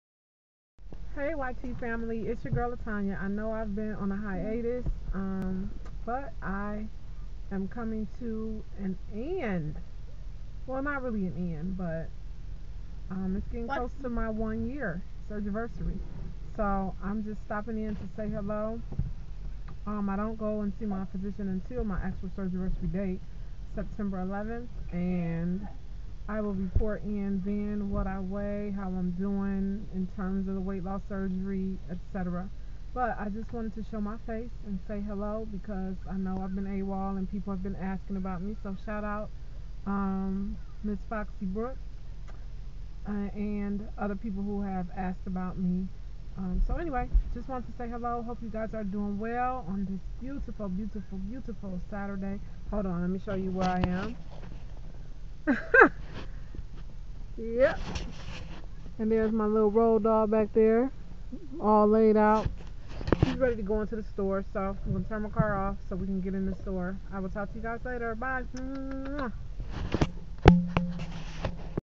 Checking in...it's been a minute
Hey all...just checking in as promised to TONI (changingb4youreyes) and Stephanie (msfoxxybrooks??)  It's almost my surgerversary and I'll be back with a 1 year update.